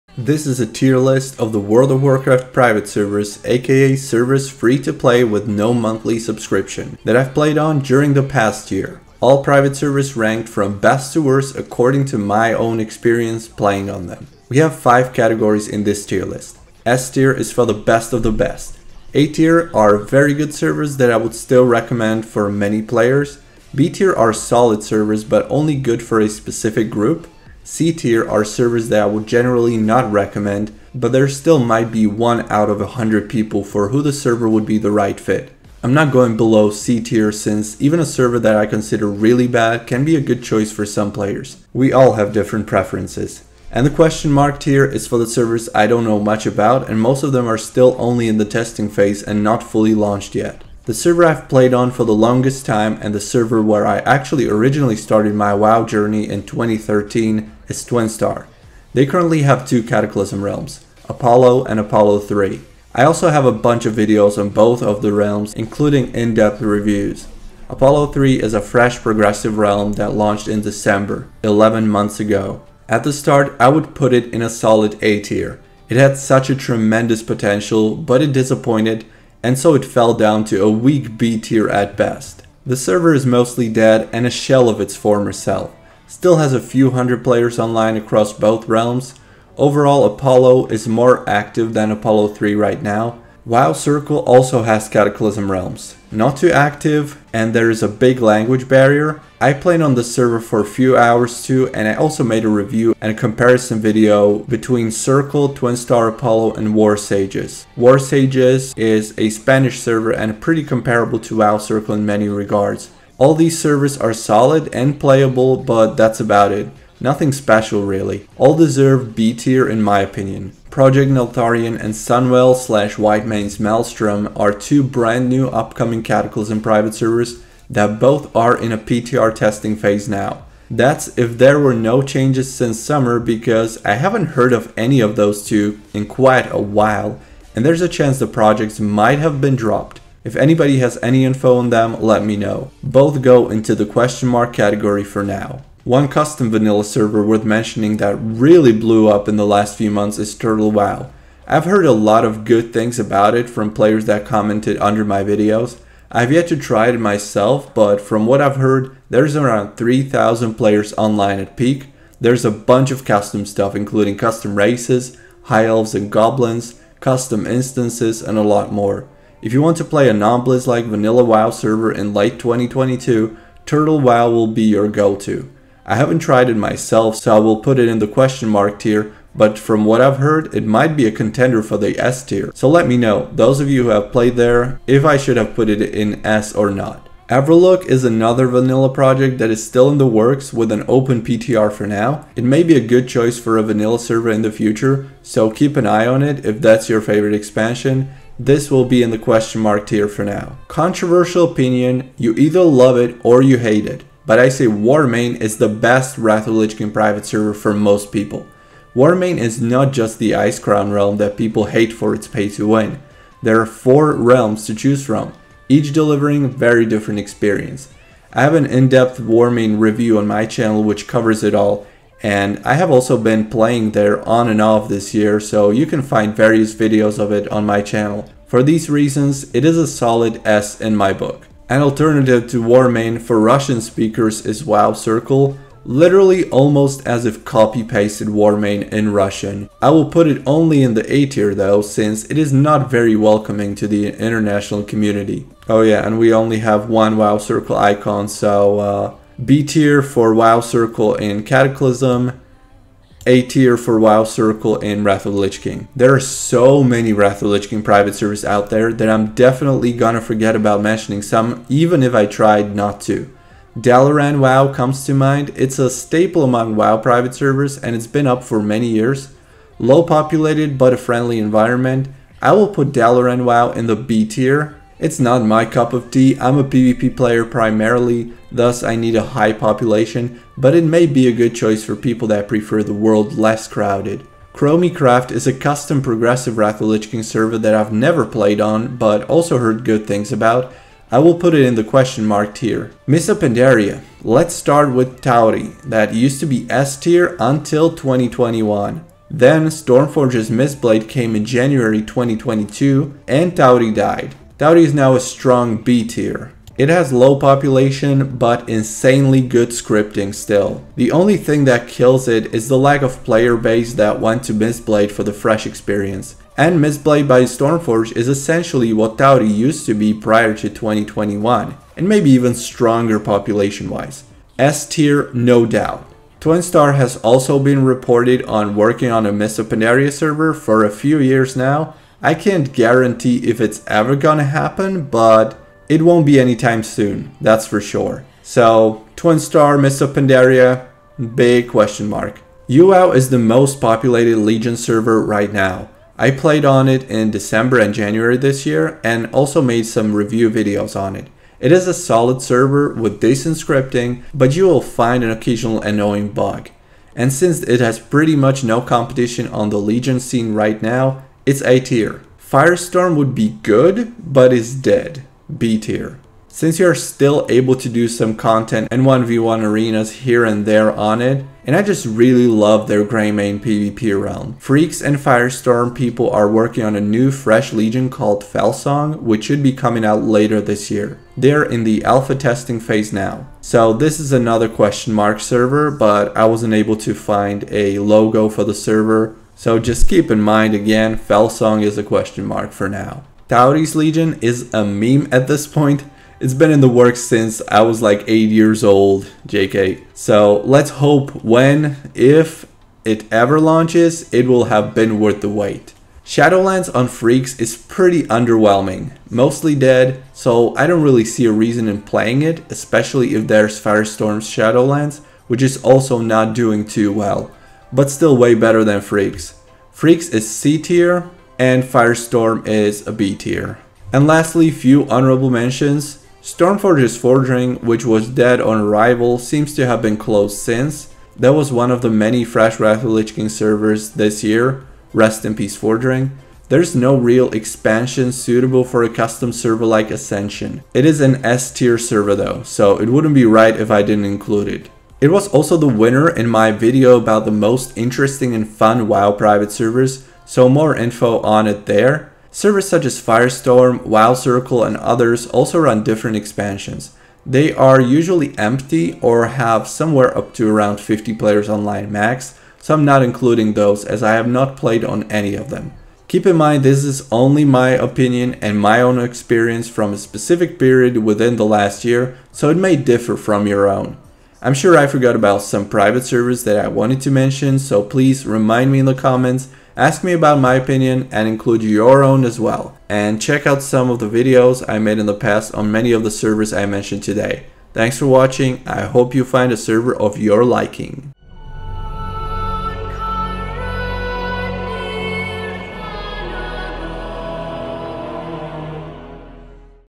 World of Warcraft Private Server TIER LIST
If you want to know which free WoW servers are the BEST and the WORST - you have come to the right place. This is the Ultimate Tier List for the biggest and most well-known World of Wacraft private servers. By Meytrix - the biggest private server fan.